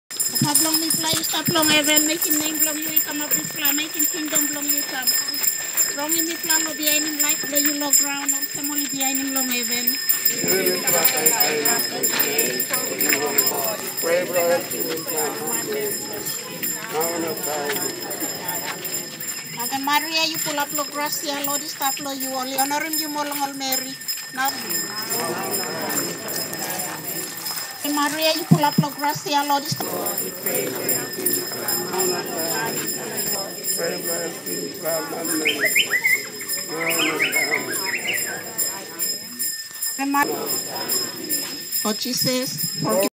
Catholic Generation of Power
The procession of The Holy Eucharist By the parishioners of Rivo  Catholic  Church North Coast of Madang , PNG  .